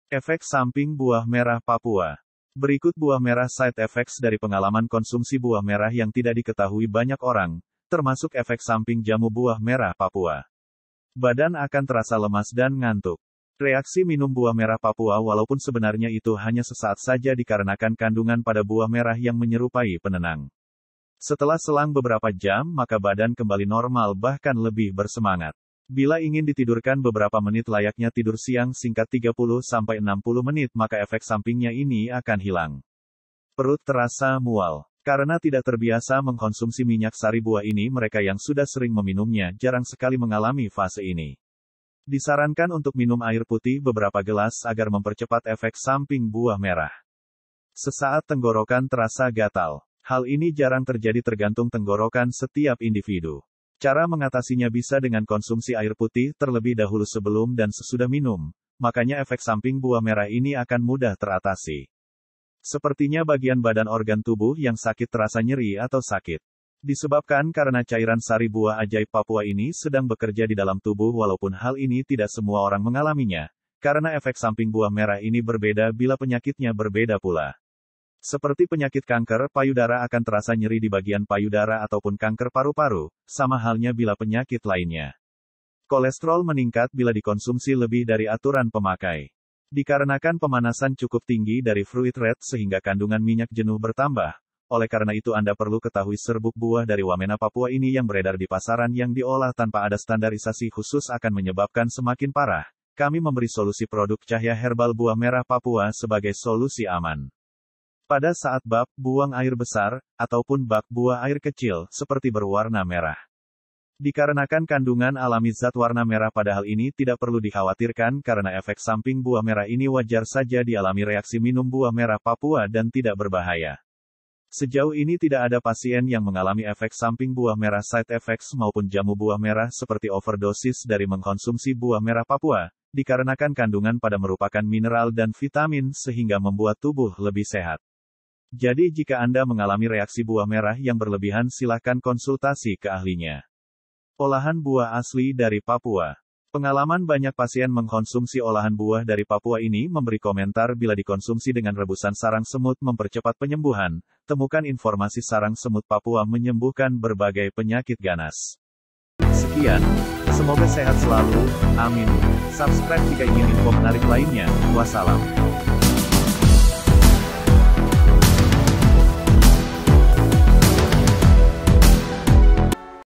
EFEK SAMPING BUAH MERAH PAPUA
Berikut buah merah side effects dari pengalaman konsumsi buah merah yang tidak diketahui banyak orang, termasuk efek samping jamu buah merah papua :
1. Badan akan terasa lemas & ngantuk.
reaksi minum buah merah papua walaupun sebenarnya itu hanya sesaat saja dikarenakan kandungan pada Buah Merah yang menyerupai penenang. setelah selang beberapa jam maka badan kembali normal bahkan lebih bersemangat. Bila ingin ditidurkan beberapa menit layaknya tidur siang singkat 30-60 menit maka efek sampingnya ini akan hilang.
2. Perut terasa mual
karena tidak terbiasa mengkonsumsi minyak sari buah ini mereka yang sudah sering meminumnya jarang sekali mengalami fase ini. Disarankan untuk minum air putih beberapa gelas agar mempercepat efek samping buah merah.
3. Sesaat Tenggorokan terasa gatal,
Hal ini Jarang terjadi tergantung tenggorokan setiap individu cara mengatasinya bisa dengan konsumsi air putih terlebih dahulu sebelum dan sesudah minum, makanya efek samping buah merah ini akan mudah teratasi.
4. Sepertinya bagian badan organ tubuh yang sakit terasa nyeri atau sakit,
disebabkan karena cairan sari buah ajaib papua ini sedang bekerja didalam tubuh walaupun hal ini tidak semua orang mengalaminya, karena efek samping buah merah ini berbeda bila penyakitnya berbeda pula. Seperti penyakit kanker payudara akan terasa nyeri dibagian payudara ataupun kanker paru-paru, sama halnya bila penyakit lainnya.
5. Kolestrol meningkat bila dikonsumsi lebih dari aturan pemakai,
dikarenakan pemanasan cukup tinggi dari fruit red sehingga kandungan minyak jenuh bertambah, oleh karena itu anda perlu ketahui serbuk buah dari Wamena Papua ini yang beredar dipasaran yang di olah tanpa ada standarisasi khusus akan menyebabkan semakin parah, kami memberi solusi Produk Cahya Herbal Buah Merah Papua sebagai solusi Aman.
6. Pada Saat BAB (Buang Air Besar) ataupun BAK (Buah Air Kecil) seperti berwarna merah,
dikarenakan kandungan alami zat warna merah padahal ini tidak perlu dikhawatirkan karena efek samping buah merah ini wajar saja dialami reaksi minum buah merah papua dan tidak berbahaya.